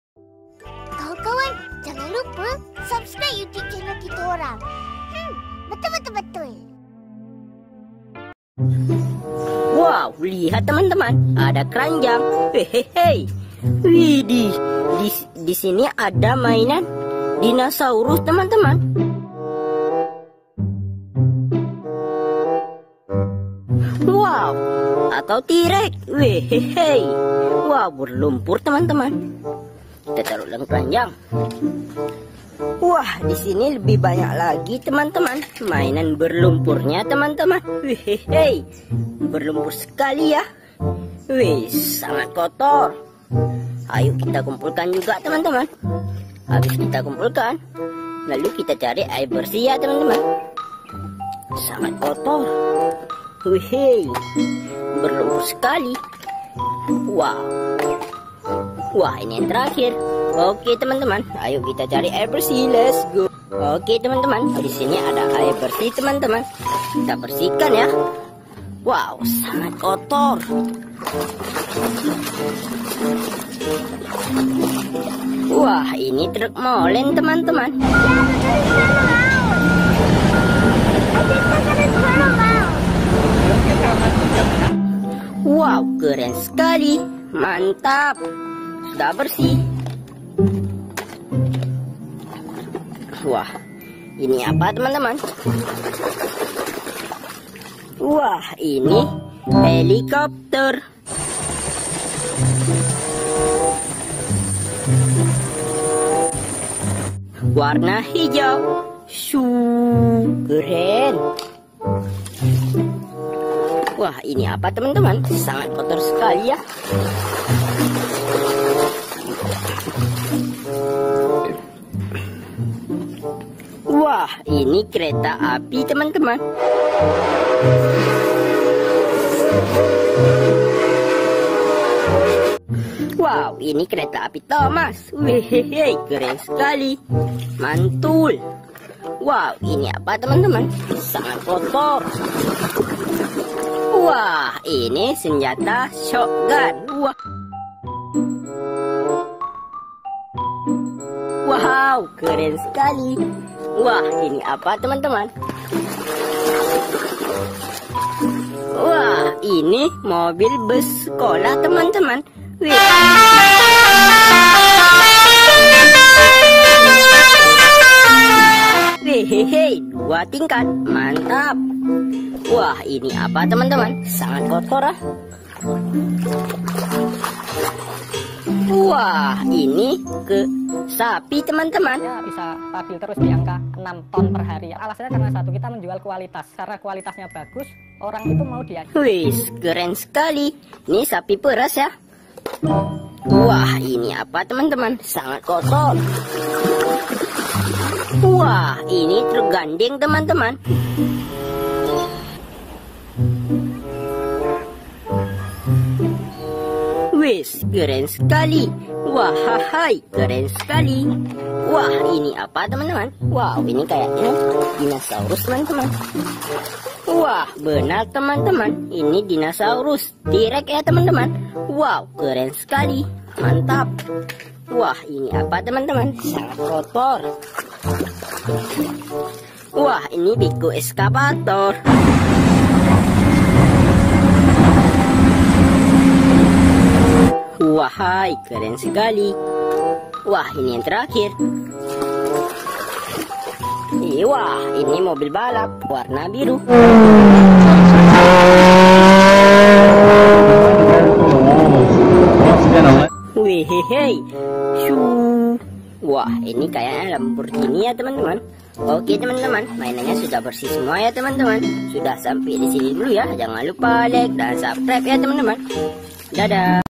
Cara Mudah Membersihkan Mainan Anda: Helikopter, Truk Molen, Bus Tingkat, Mobil Balap Dan Kereta Api
Hai teman-teman! Dalam video kali ini, kami akan memandu Anda melalui proses membersihkan mainan favorit Anda, termasuk helikopter, truk molen, bus tingkat, mobil balap, kereta api, dan beko excavator. Kami akan menunjukkan cara membersihkan mainan dengan benar agar tetap terlihat baru dan bersih sepanjang waktu. Ikuti langkah-langkah kami dan jangan lupa untuk berbagi tips membersihkan mainan Anda di kolom komentar. Ayo mulai membersihkan mainan sekarang!